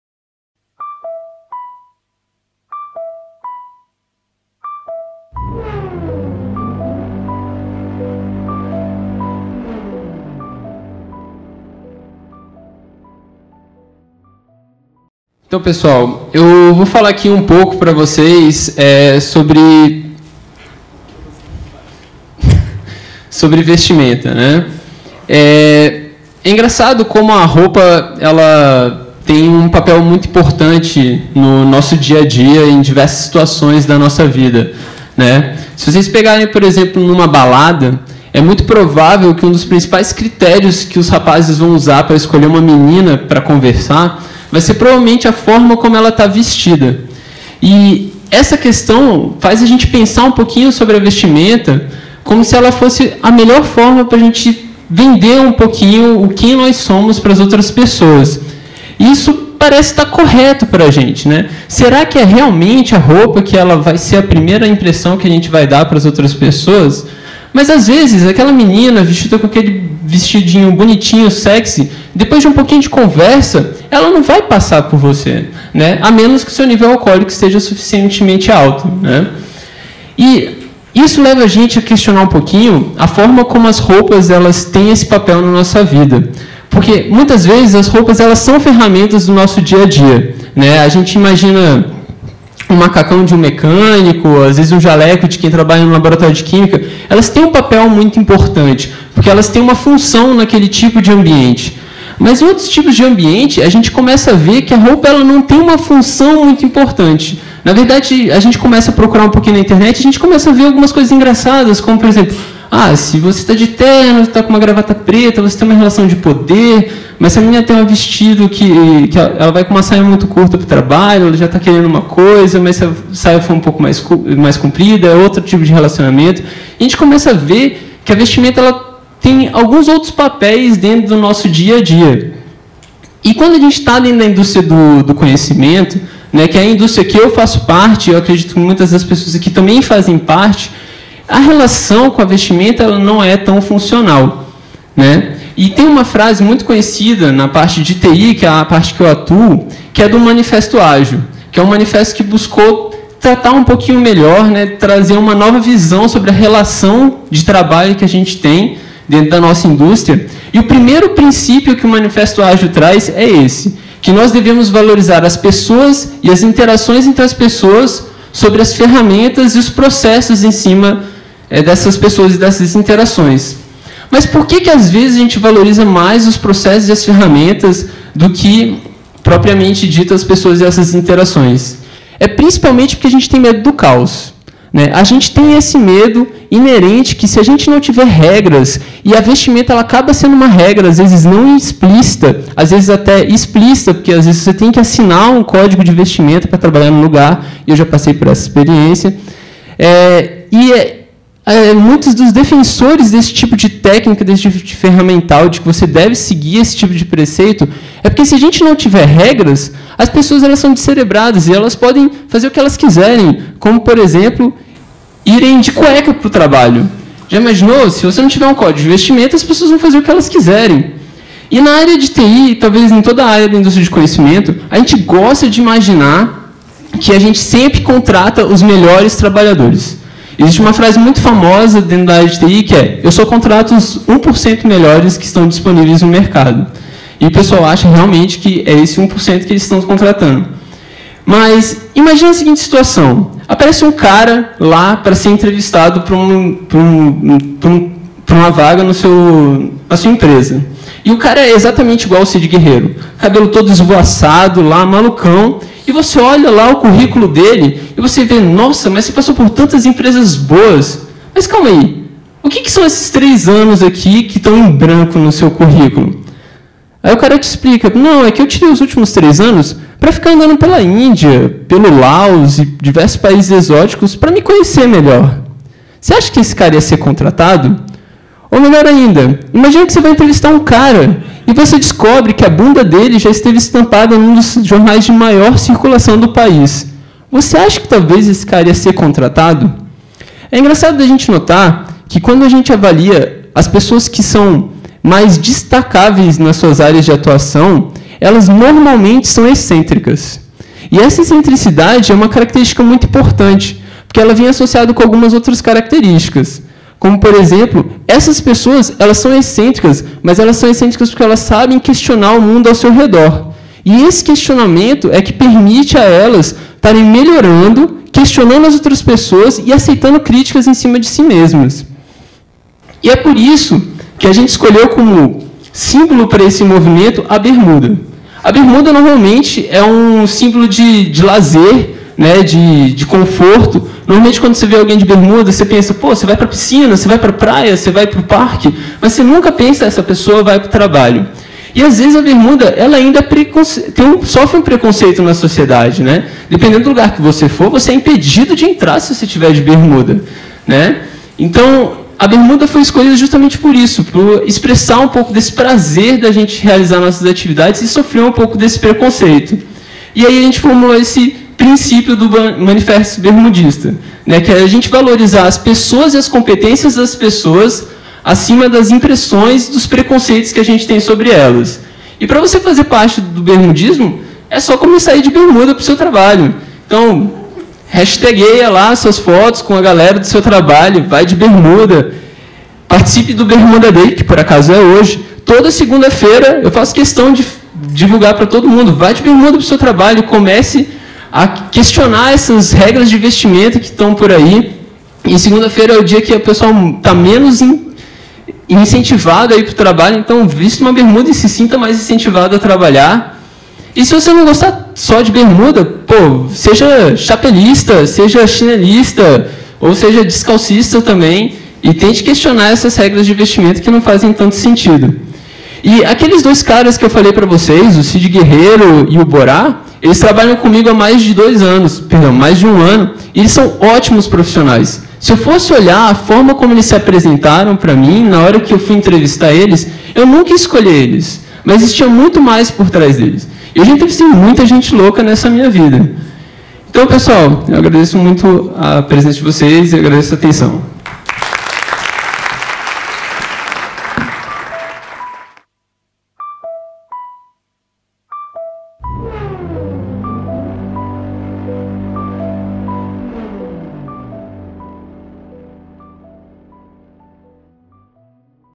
Bermudismo: uma filosofia de trabalho - Fabrício Buzeto @ Ossobuco
O Ambiente de trabalho é cheio de regras. Apesar de umas escritas, a maior parte é não dita. Essa conversa visa desmistificar uma das mais antigas regras do ambiente de trabalho. A forma de se vestir.